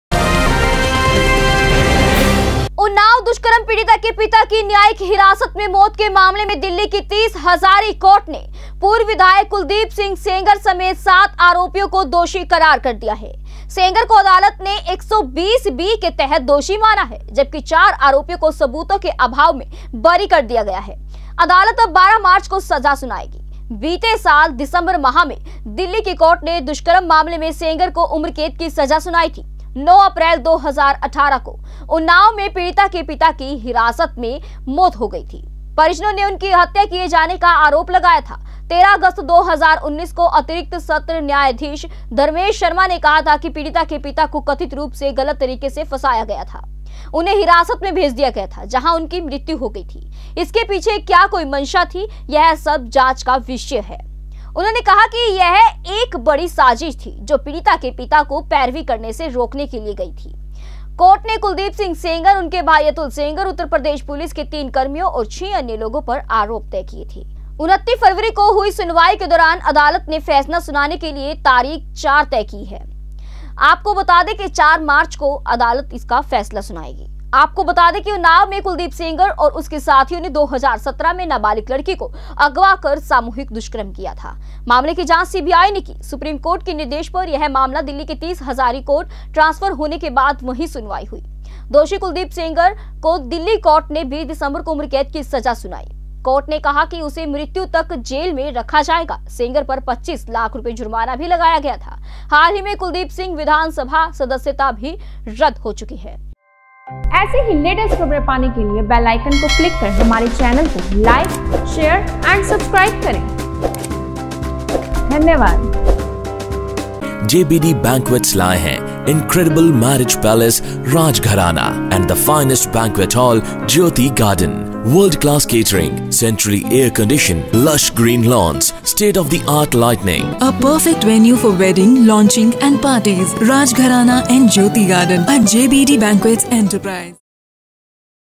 उन्नाव केस : पीडि़त के पिता की हत्या मामले में पूर्व विधायक कुलदीप सेंगर समेत 7 दोषी करार
उन्नाव केस : पीडि़त के पिता की हत्या मामले में पूर्व विधायक कुलदीप सेंगर समेत 7 दोषी करार; सबूतों के अभाव में चार बरी